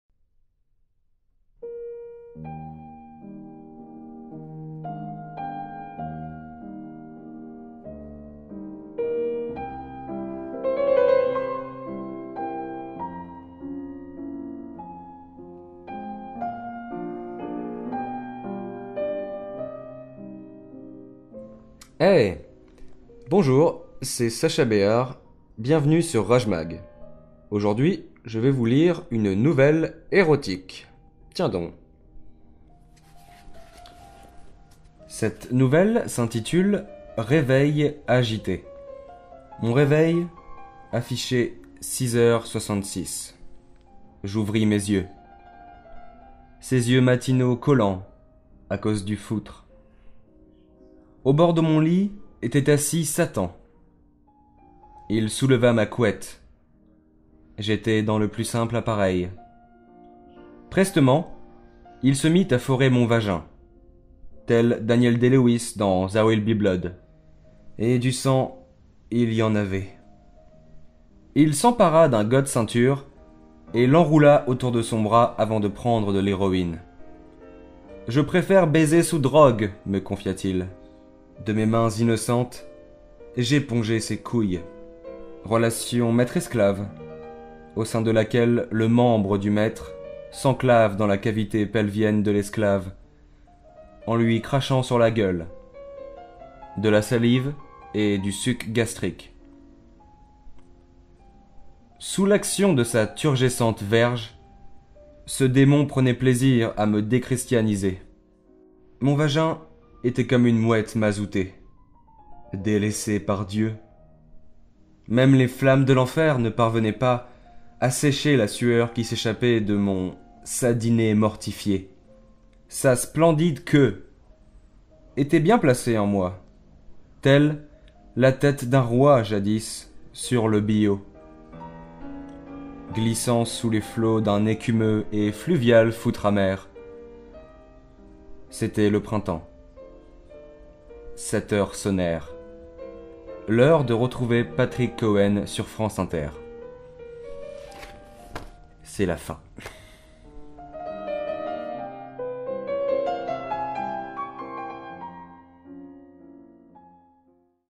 Sacha Béhar : Réveil agité
Les nouvelles érotiques de Sacha Béhar : Réveil agité

Laissez Sacha Béhar vous entraîner dans son univers fantasmatique avec une deuxième nouvelle érotique diablement excitante, dont la lecture a été filmée dans nos studios. Bons émois...